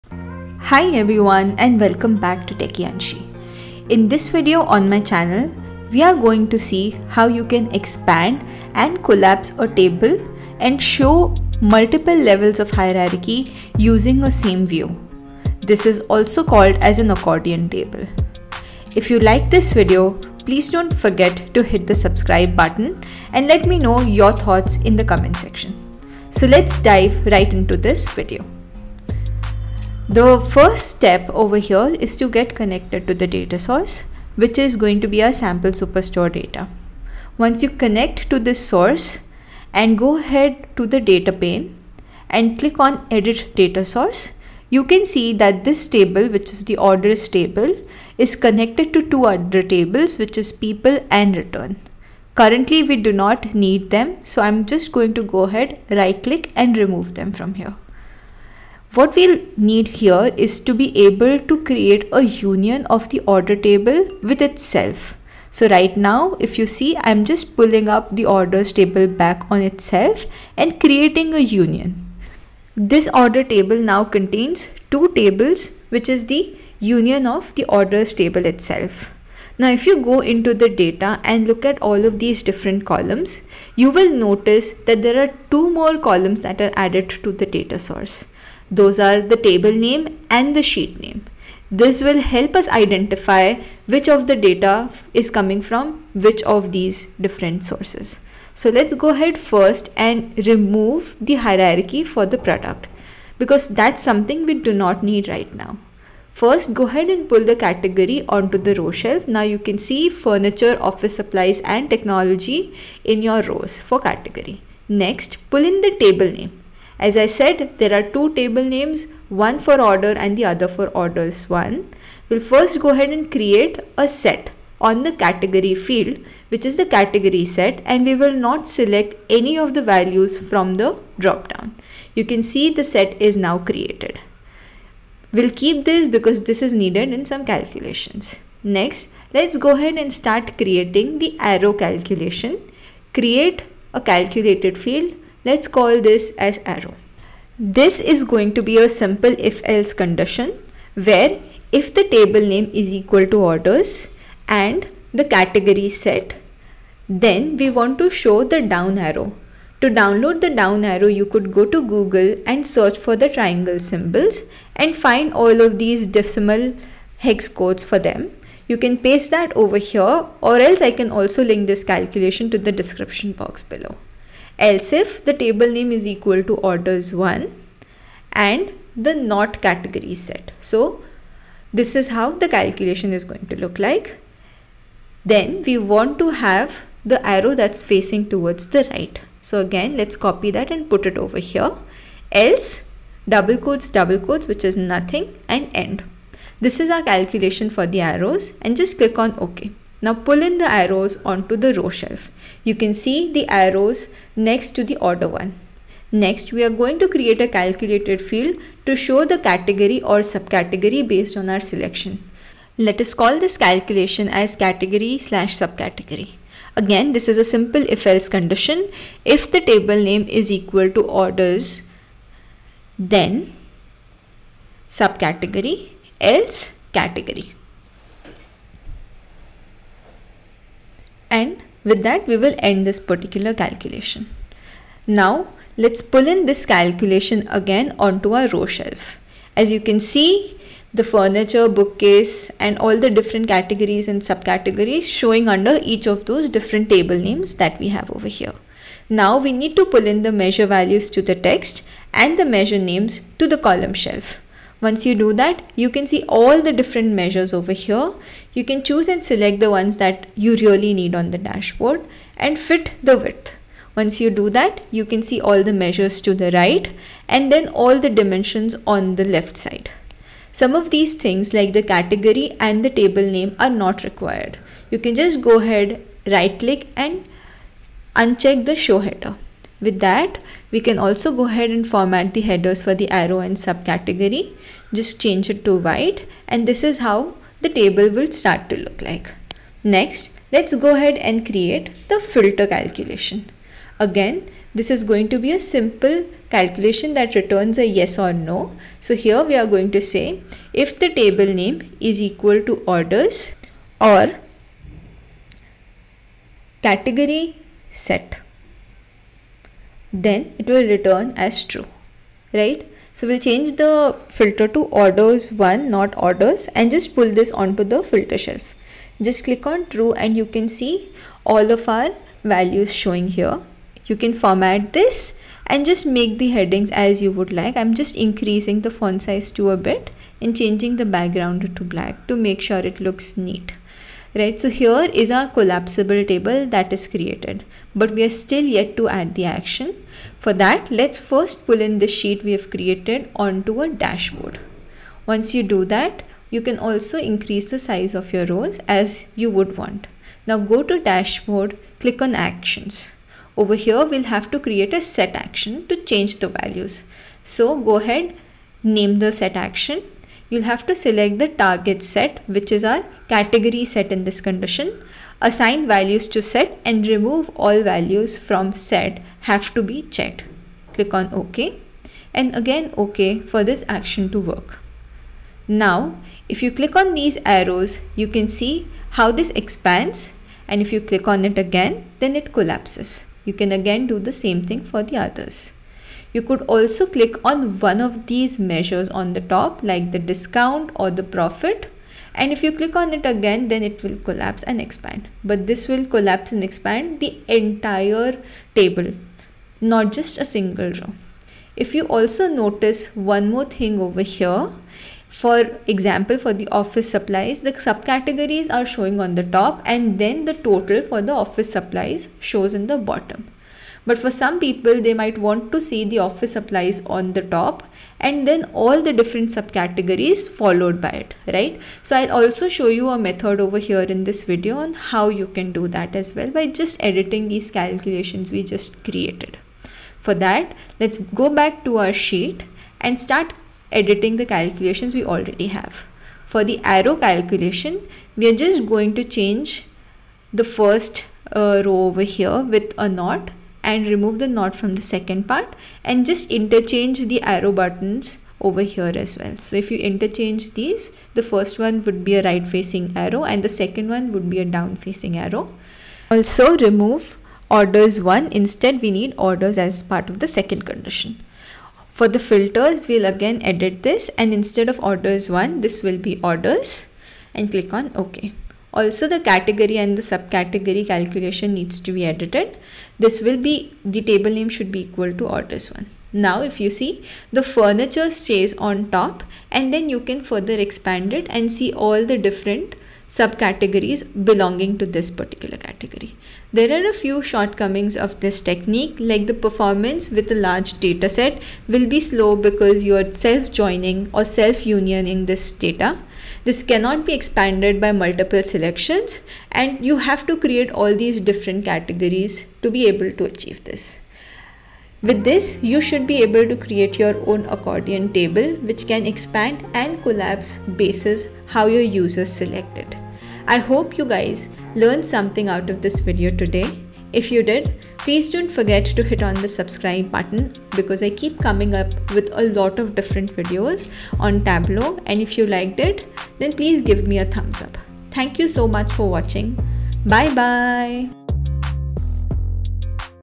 How to Create Accordion Table in Tableau | Expanding & Collapsing Table using Arrows in Tableau
This video will enable you to create a Table which can expand and collapse using the Arrow buttons, where you can see the Categories and Subcategories in the same view as you would like

For this you will need a few calculations I am listing them down here for your reference:
1) Arrow Calculation
IF [Table Name]= "Orders"
AND NOT [Category Set]
THEN  "▶"
ELSEIF [Table Name]="Orders"
AND [Category Set]
THEN "▼"
ELSE ""
END

2) Category/SubCategory
IF [Table Name]="Orders1" THEN [Sub-Category]
ELSE [Category]
END

3) Filter
[Table Name]="Orders" OR [Category Set]

Please let me know your thoughts on this video and some future video suggestions

Happy Learning!!